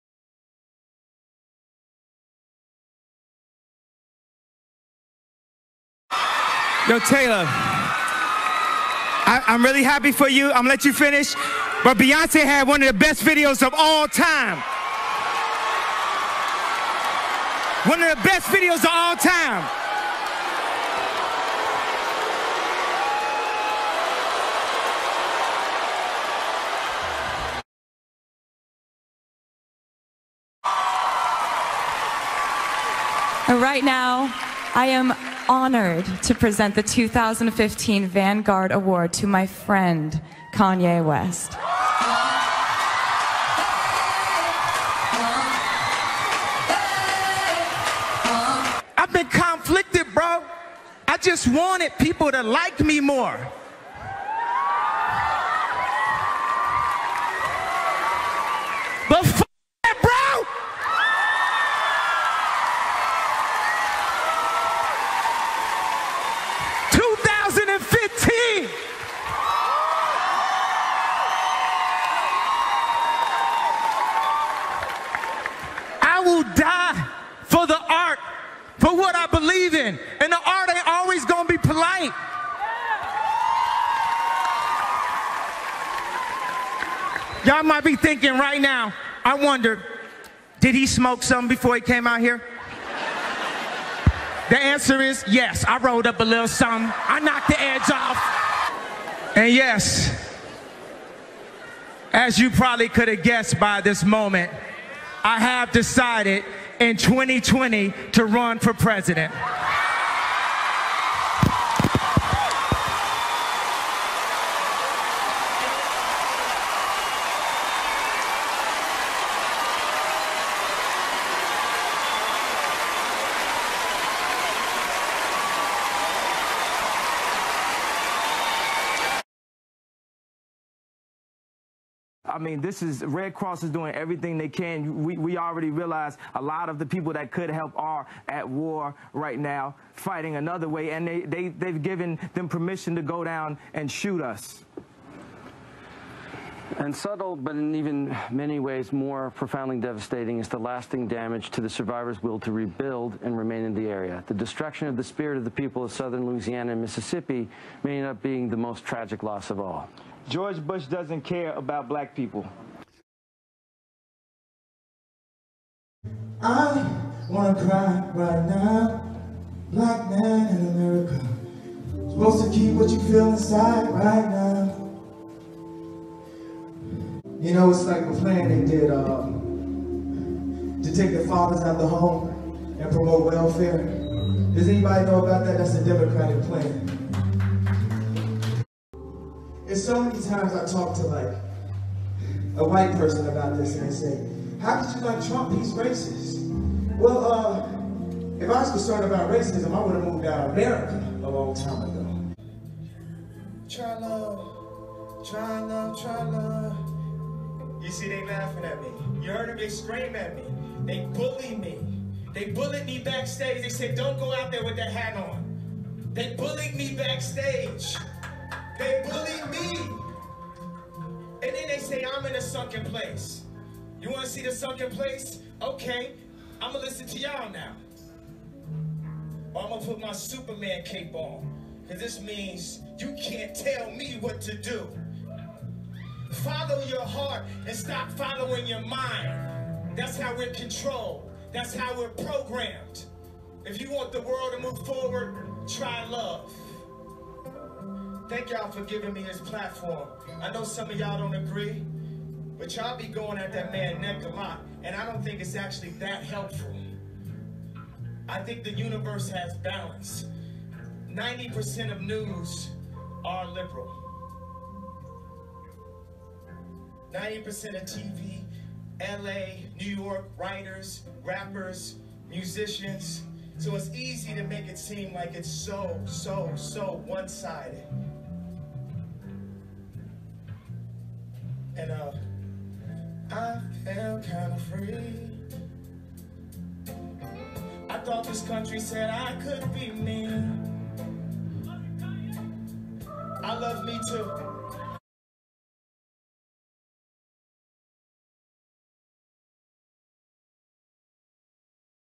Kanye West's Craziest And Cringe-Worthy Moments
Being Yeezy ain't easy! Kanye West has never shied away from controversy throughout his career. In fact, he has a habit of stirring the pot.
We've compiled a list of the five most humiliating and cringe-worthy Kanye moments.